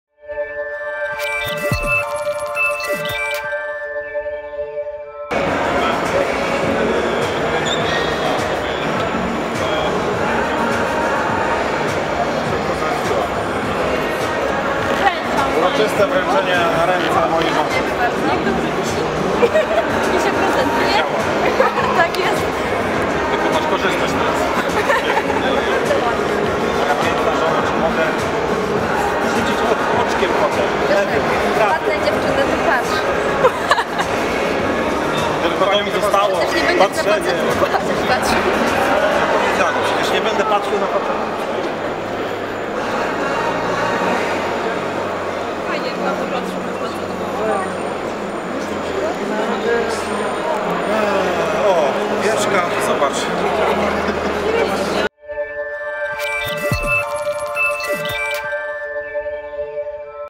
Ania i Grzegorz Bardowscy otrzymali Kalendarz Rolniczki 2018
Na Targach Polagra Premiery 2018, Gosia - Rolniczka z okładki naszego kalendarza 2018, podpisała i przekazała Państwu Bardowskim Kalendarz Rolniczki 2018.